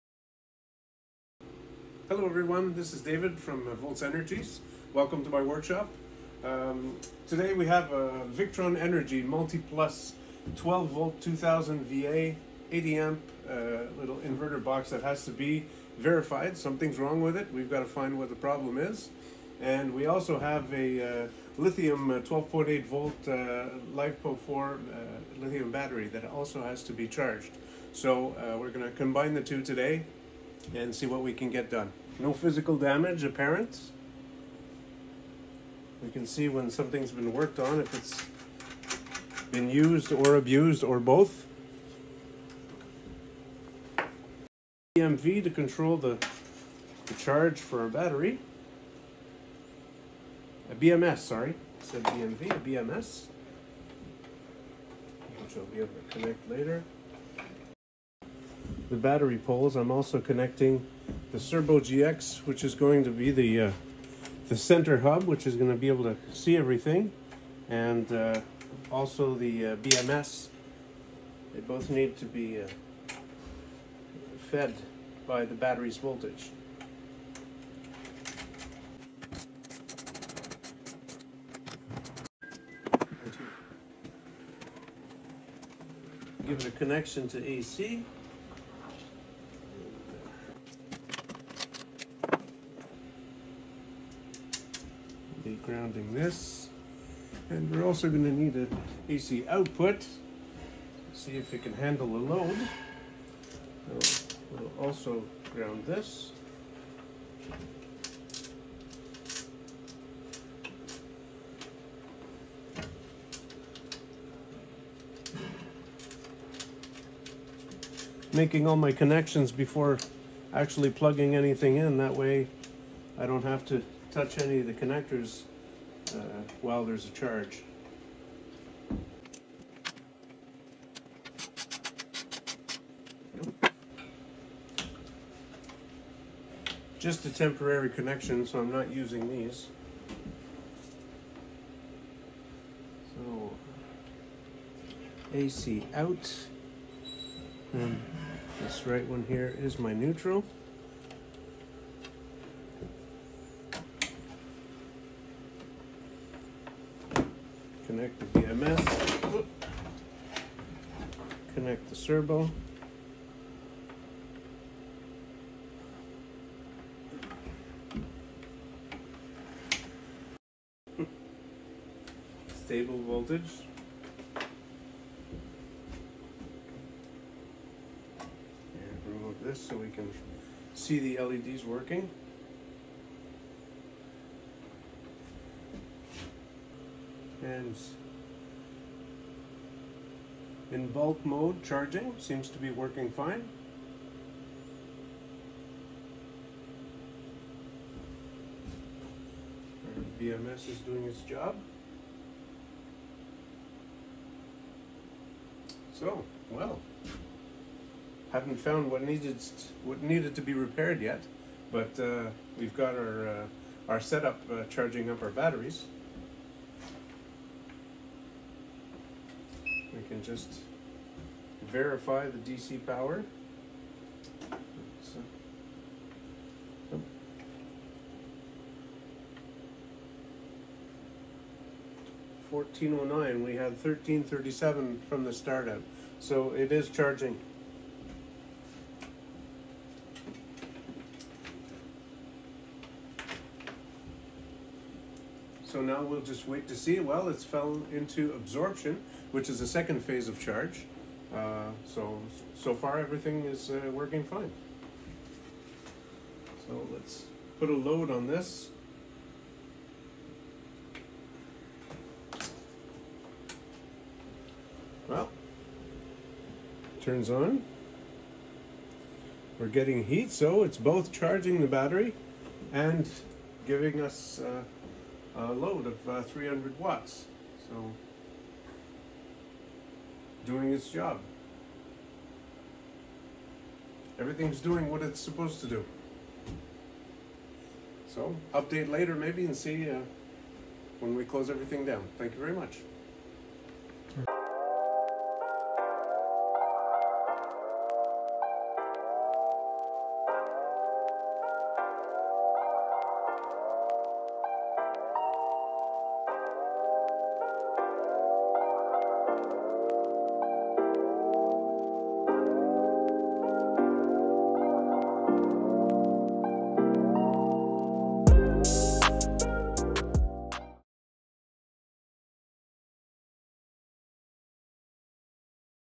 Volts Workshop | Troubleshooting Victron Energy Products
Join us inside the Volts Energies workshop as we put the @VictronEnergyBV  Energy MultiPlus-II Inverter/Charger to the test! ⚡🔋 We’re pairing it with a Victron 12.8V Lithium Battery and the Cerbo GX monitoring system to see how this powerful setup performs.

Victron Energy is known for its high-quality solar and energy storage solutions, and we’re excited to share our hands-on experience! Whether you’re designing an off-grid system, an RV setup, or a backup power solution, this test will help you understand how these components work together.